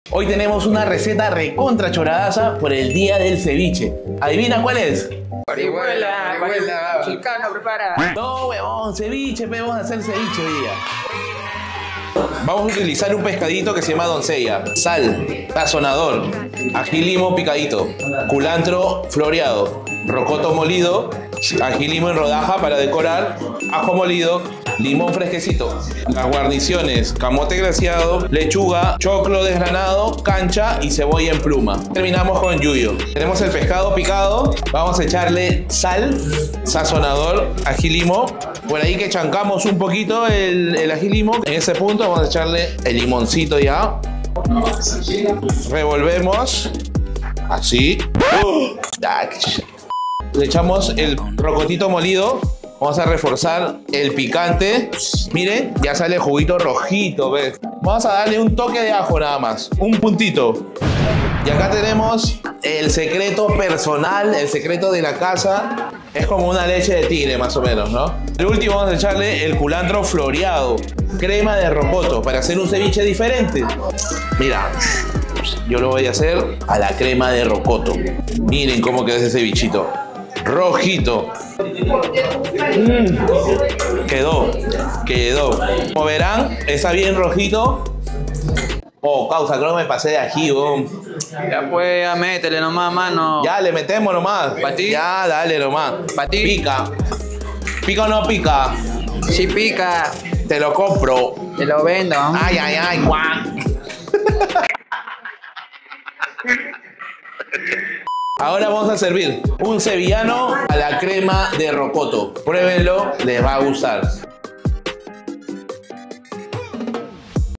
Ceviche al rocoto por el día del ceviche
Feliz día del ceviche a todos los chalacos, amigos, colegas cocineros y seguidores🐟🐟. Aquí les dejo una recetita de un cevichito bien achorado🤙🤙 👨‍🍳LA ALDEA DE GASTON y 🎥Totem Agency @destacar